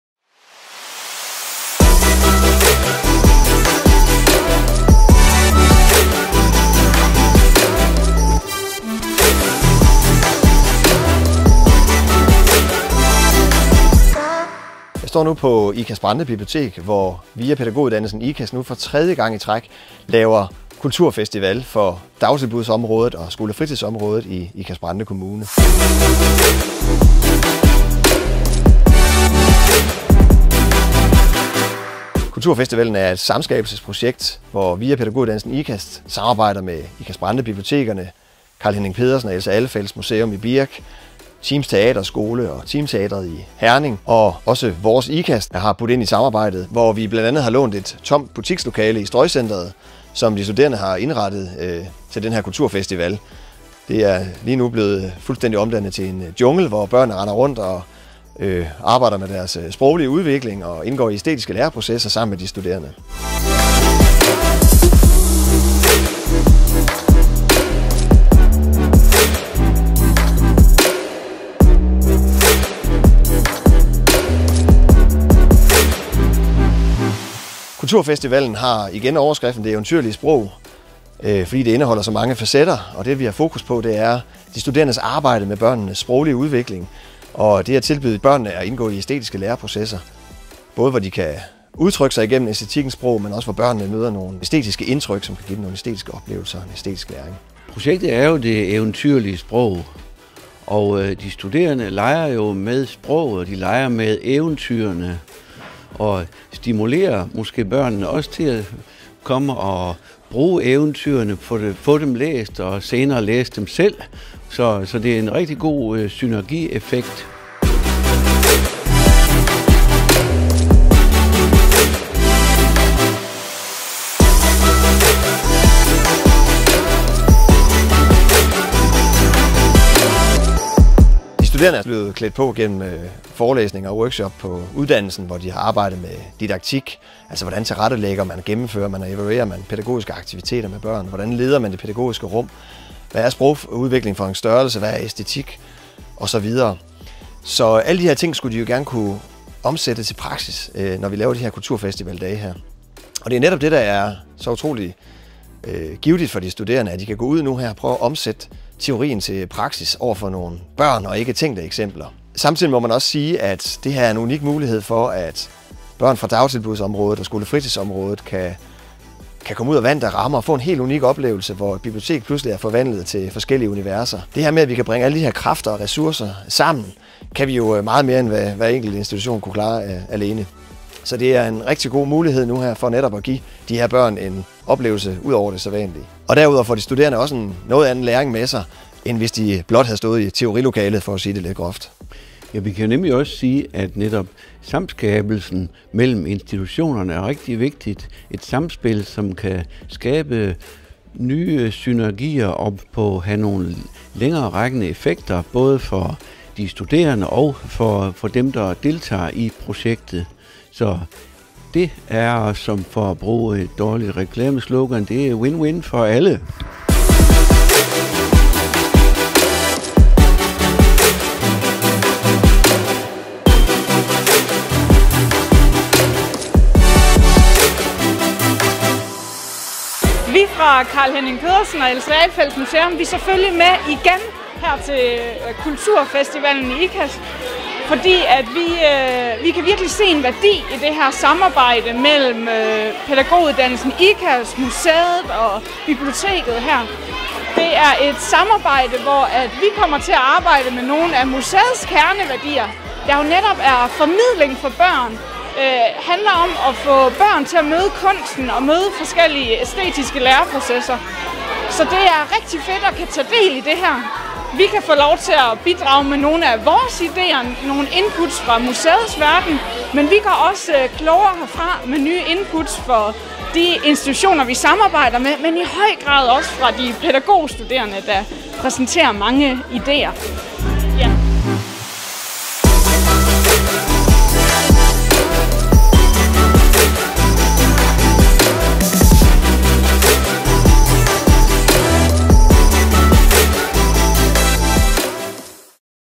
Kulturfestival (november) 2018
(HUSK LYD) Onsdag d. 21 og torsdag d. 22. november afviklede hold PI18S fra Pædagoguddannelsen i Ikast Kulturfetival for dagtilbud og skolefritidsområdet i Ikast-Brande Kommune. Dette skete i tæt samskabelse med vores samarbejdspartnere Teams Teaterskole, Team Teatret, Vores Ikast, Carl-Henning Pedersen & Else Alfelts Museum og Ikast-Brande Bibliotek. Se mere i videoen her. Del og like gerne. Tak til alle medvirkende for en fantastisk festival.

(For bedre kvalitet tryk HD)

Produceret af Jakob Bjerre: Lektor, VIA Pædagoguddannelsen Ikast